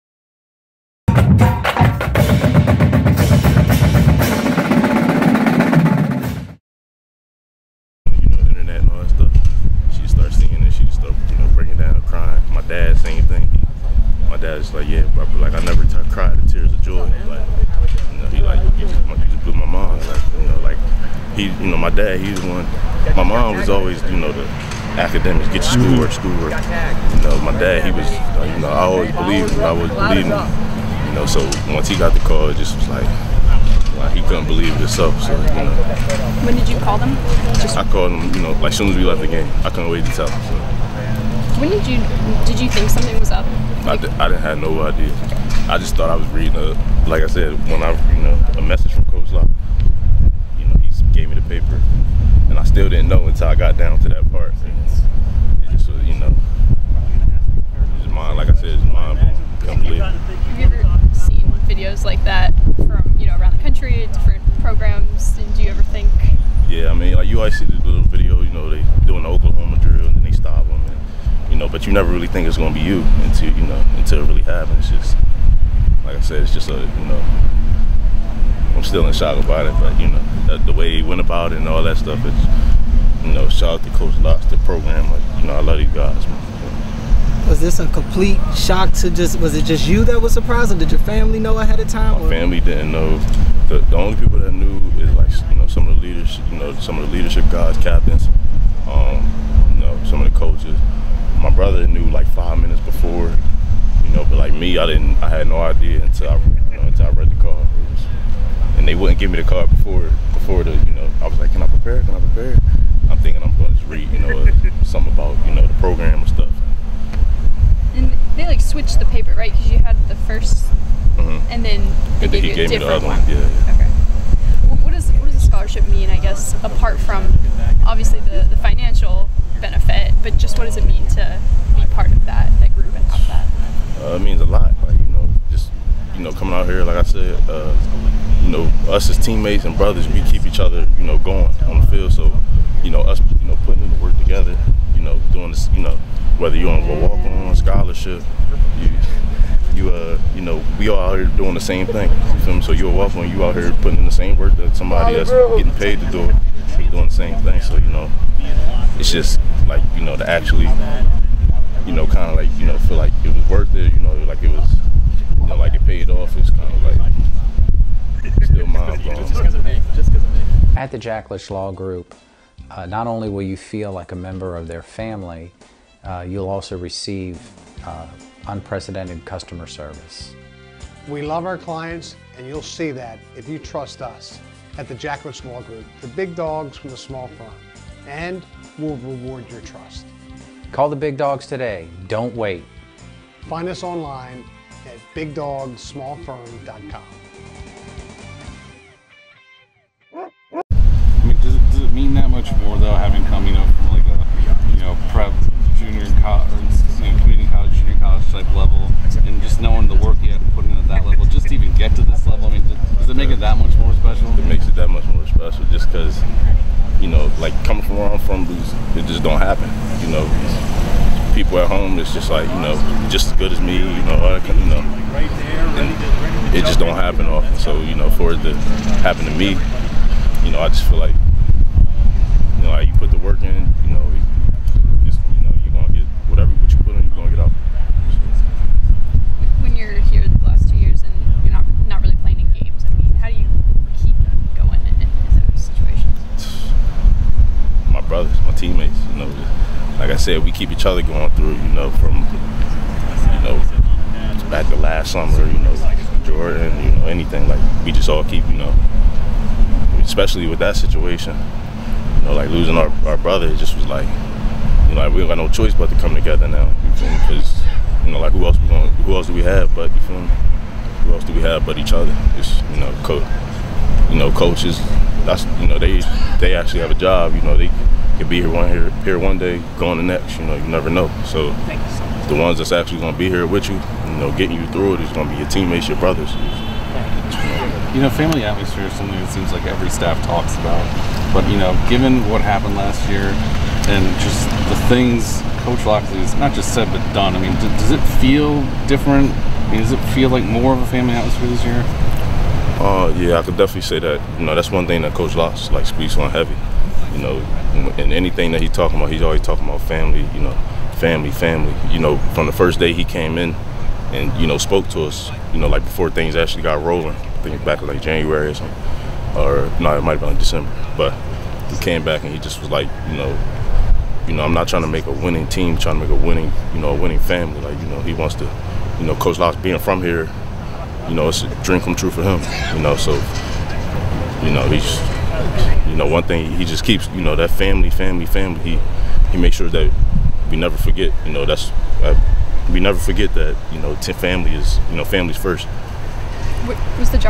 Bruce Miller Fall Camp #16 2019
Bruce Miller talks about getting a scholarship at Fall camp